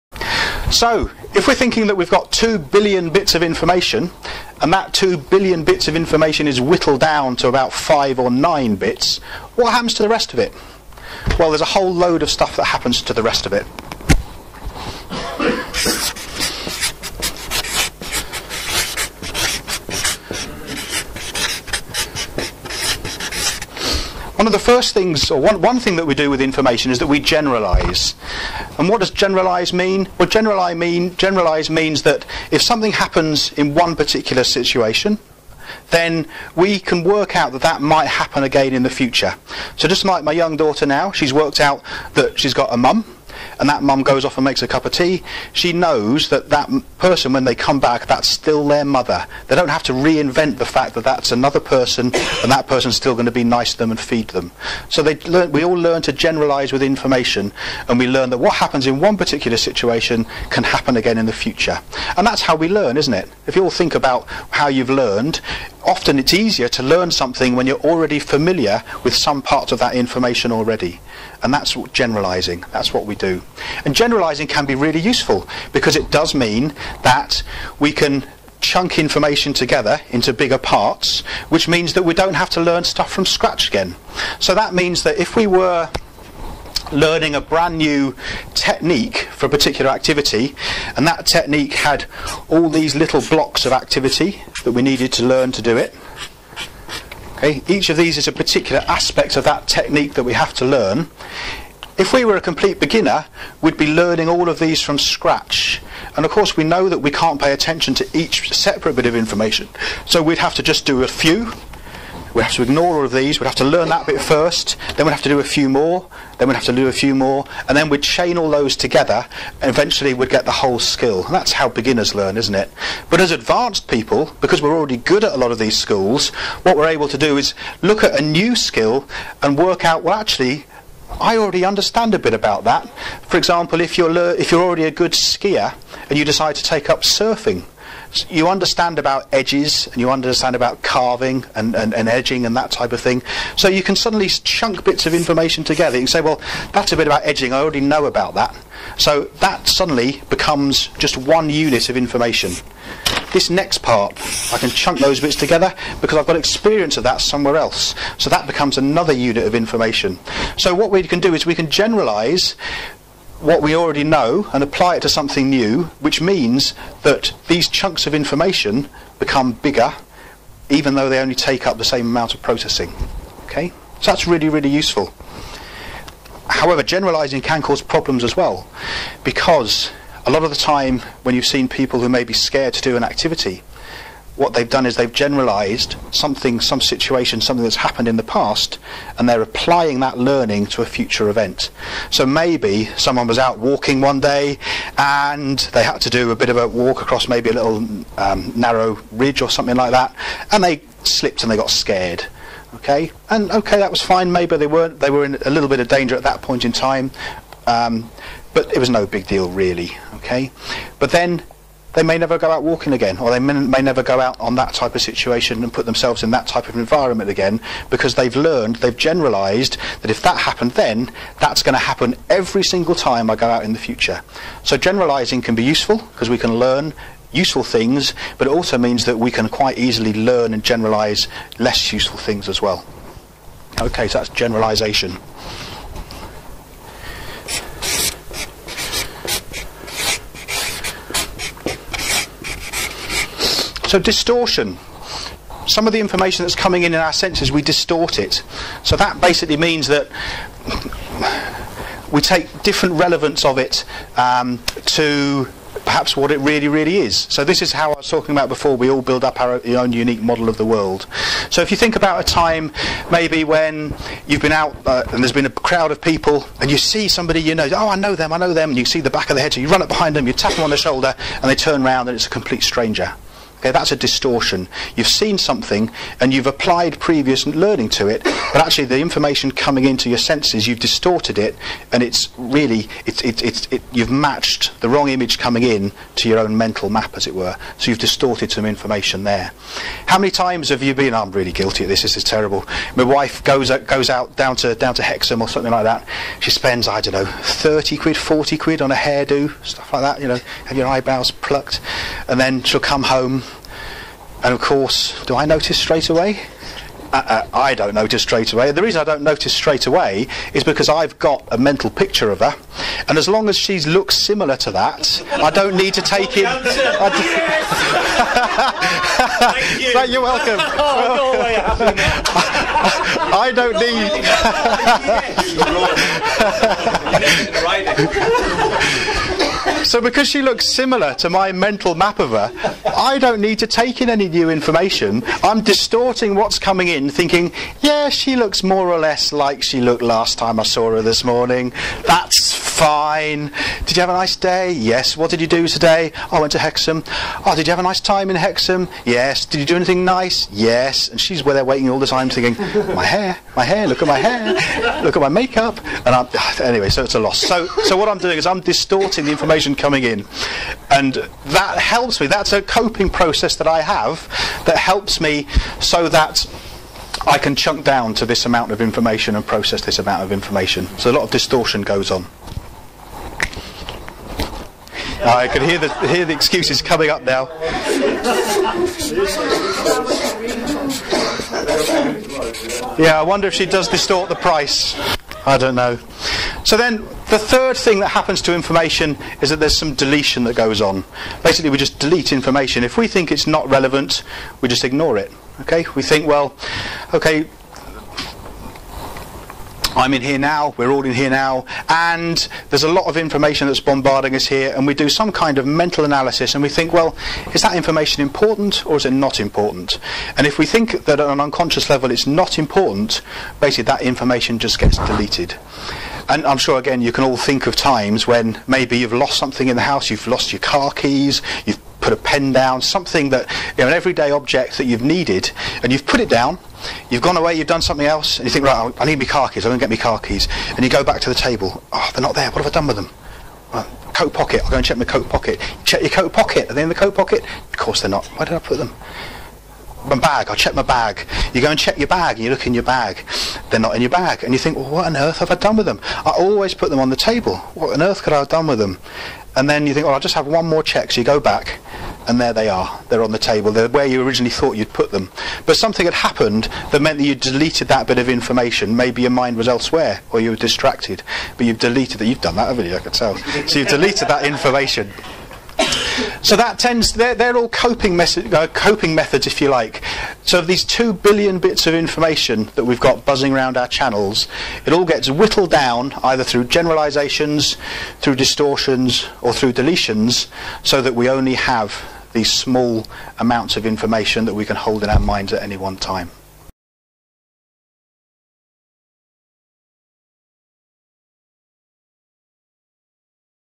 Information Processing
This clip, from the 'NLP for Sports Excellence' training, explains how the processes on Generalisation, Deletion and Distortion affect the way we process information.